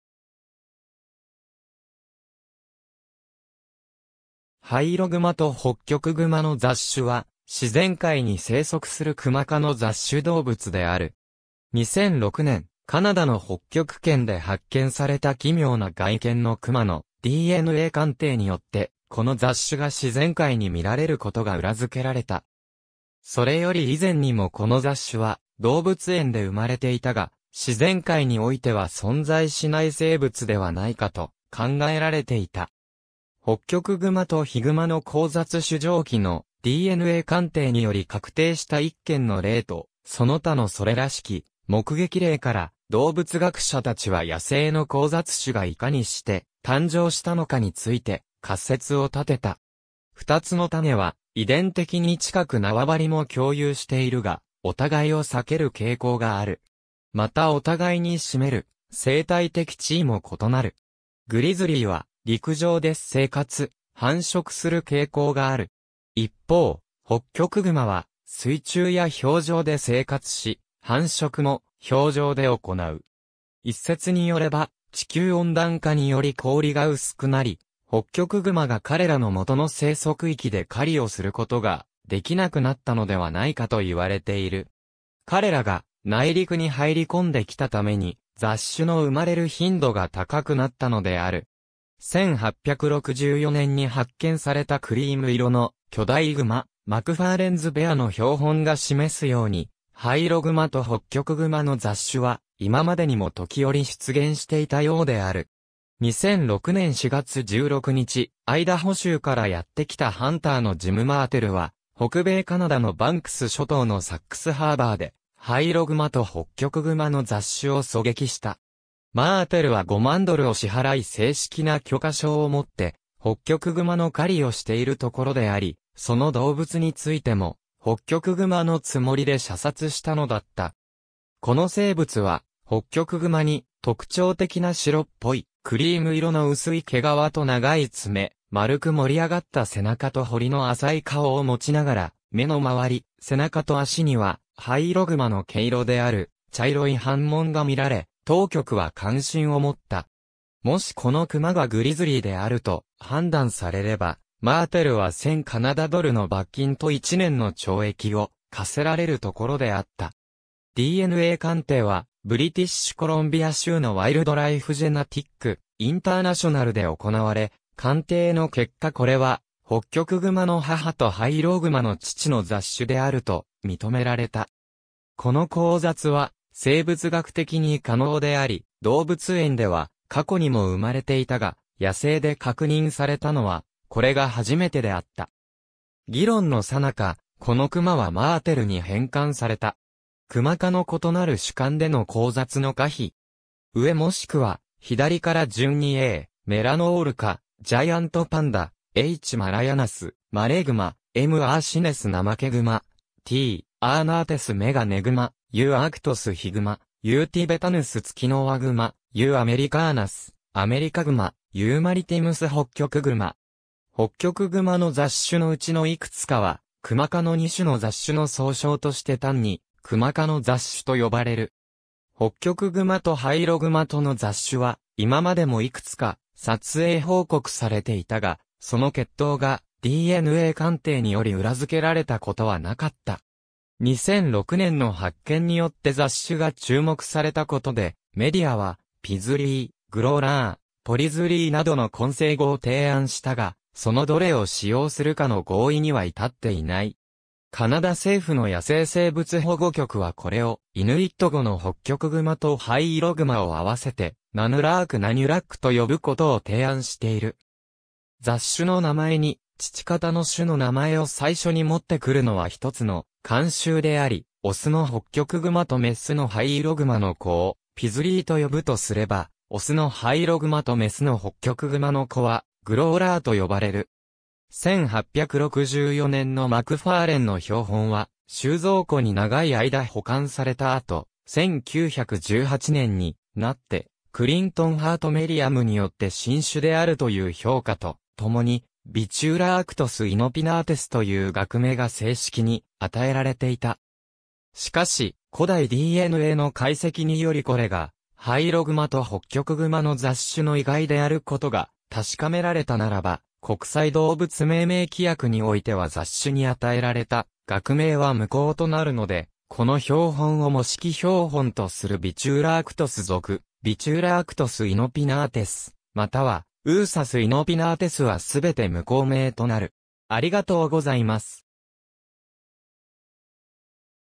ハイイログマとホッキョクグマの雑種
ハイイログマとホッキョクグマの雑種は、自然界に生息するクマ科の雑種動物である。
2006年、カナダの北極圏で発見された奇妙な外見のクマのDNA鑑定によって、この雑種が自然界に見られることが裏付けられた。
それより以前にもこの雑種は動物園で生まれていたが、自然界においては存在しない生物ではないかと考えられていた。
ホッキョクグマとヒグマの交雑種（ロスチャイルド動物学博物館） 上記のDNA鑑定により確定した一件の例とその他のそれらしき目撃例から、動物学者たちは野生の交雑種がいかにして誕生したのかについて仮説を立てた。
2つの種は遺伝的に近く縄張りも共有しているが、お互いを避ける傾向がある。
またお互いに占める生態的地位も異なる。
グリズリー（また他のアラスカに生息するアラスカアカヒグマなどは全てヒグマ Ursus arctos の一種）は陸上で生活、繁殖する傾向がある。
一方、ホッキョクグマは水中や氷上で生活し、繁殖も氷上で行う。
一説によれば地球温暖化により氷が薄くなり、ホッキョクグマが彼らのもとの生息域で狩りをすることができなくなったのではないかと言われている。
彼らが内陸に入り込んできたために、雑種の生まれる頻度が高くなったのである。
1864年に発見されたクリーム色の巨大グマ、マクファーレンズ・ベアの標本が示すように、ハイイログマとホッキョクグマの雑種は今までにも時折出現していたようである。
2006年4月16日、アイダホ州からやってきたハンターのジム・マーテル (Jim Martell) は北米カナダのバンクス諸島のサックスハーバー (Sachs Harbour) でハイイログマとホッキョクグマの雑種を狙撃した。
マーテルは5万ドルを支払い正式な許可証を持ってホッキョクグマの狩りをしているところであり、その動物についてもホッキョクグマのつもりで射殺したのだった。
この生物はホッキョクグマに特徴的な白っぽいクリーム色の薄い毛皮と長い爪、丸く盛り上がった背中と彫りの浅い顔をもちながら、眼の周り、背中と足にはハイイログマの毛色である茶色い斑紋が見られ、当局は関心をもった。
もしこのクマがグリズリーであると判断されれば、マーテルは1000カナダドルの罰金と1年の懲役を科せられるところであった。
DNA鑑定はブリティッシュコロンビア州の Wildlife Genetic International で行われ、鑑定の結果これはホッキョクグマの母とハイイログマの父の雑種であると認められた。
この交雑は生物学的に可能であり動物園では過去にも生まれていたが、野生で確認されたのはこれが初めてであった。
議論のさ中、このクマはマーテルに返還された。
クマ科の異なる種間での交雑の可否。
上もしくは左から順に A. melanoleuca ジャイアントパンダ、H. malayanus マレーグマ、M. ursinus ナマケグマ、T. ornatus メガネグマ、U. arctos ヒグマ、U. thibetanus ツキノワグマ、U. americanus アメリカグマ、U. maritimus ホッキョクグマ。
ホッキョクグマの雑種のうちのいくつかは、クマ科の2種の雑種の総称として単にクマ科の雑種と呼ばれる。
ホッキョクグマとハイイログマ（グリズリー）との雑種は今までもいくつか撮影報告されていたが、その血統がDNA鑑定により裏付けられたことは無かった。
2006年の発見によって雑種が注目されたことで、メディアはピズリー (pizzly)、グローラー (grolar bear)、ポリズリー (polizzly) などの混成語を提案したが、そのどれを使用するかの合意には至っていない。
カナダ政府の野生生物保護局はこれをイヌイット語のホッキョクグマ（ナヌーク Nanuk）とハイイログマ（アクラーク Aklak）を合わせてナヌラーク Nanulak と呼ぶことを提案している。
雑種の名前に父方の種の名前を最初に持ってくるのはひとつの慣習であり、雄のホッキョクグマと雌のハイイログマの子を"ピズリー"と呼ぶとすれば、雄のハイイログマと雌のホッキョクグマの子は"グローラー"と呼ばれる。
1864年のマクファーレンの標本は収蔵庫に長い間保管された後、1918年になってクリントン・ハート・メリアムによって新種であるという評価とともに、Vetularctos inopinatus という学名が正式に与えられていた。
しかし、古代DNAの解析によりこれがハイイログ...